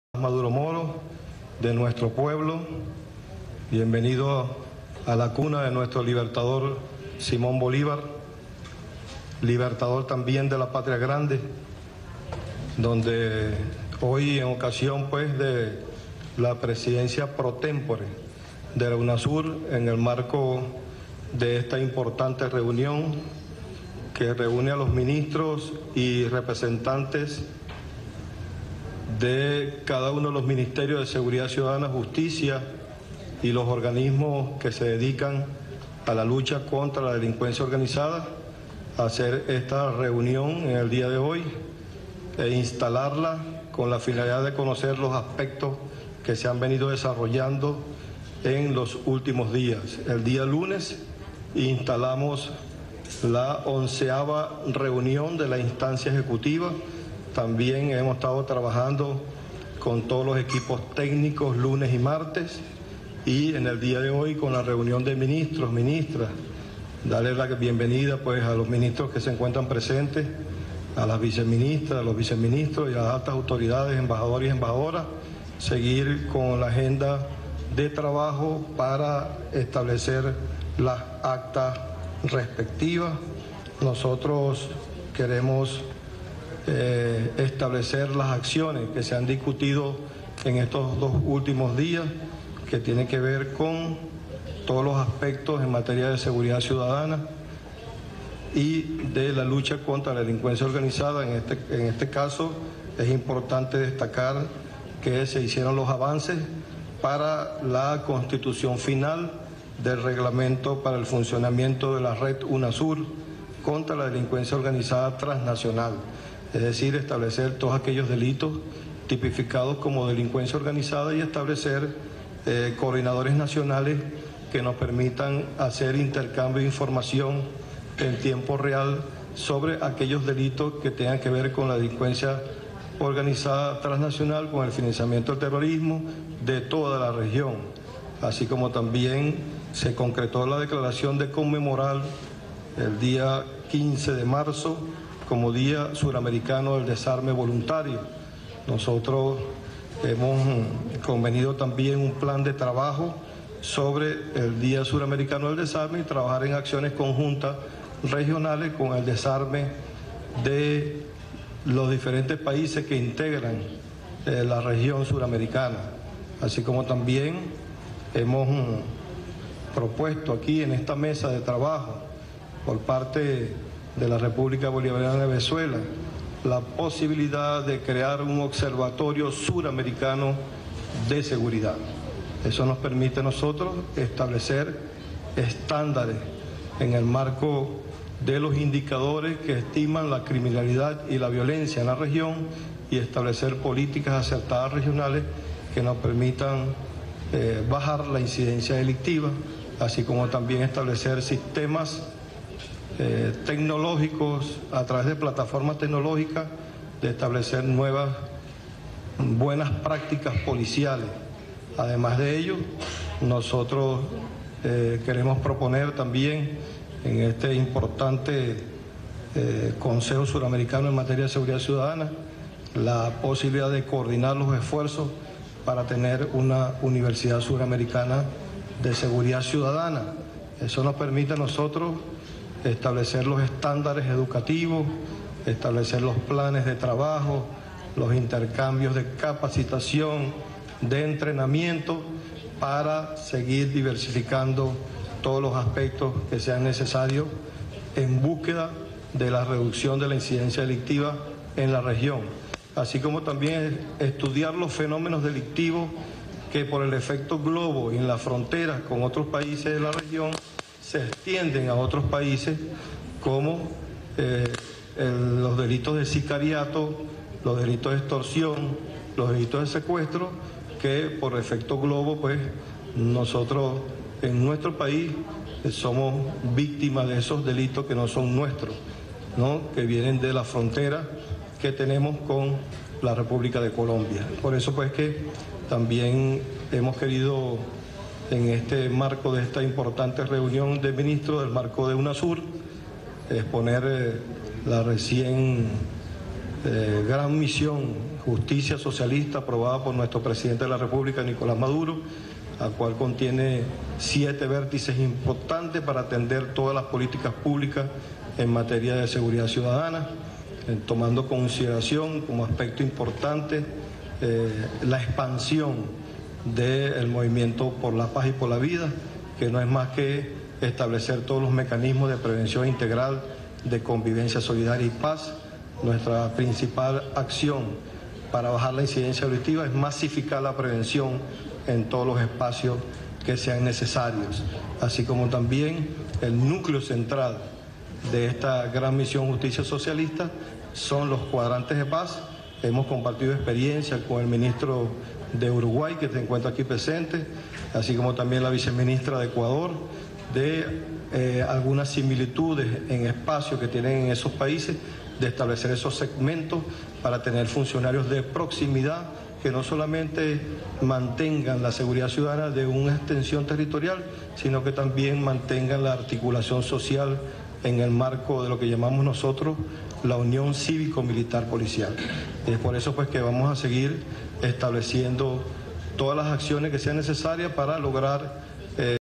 Reunión Consejo de ministros Suramericanos
Reunión Consejo de ministros Suramericanos instalada en Caracas: se concretó el 15 de marzo como el Día Suramericano del Desarme Voluntario.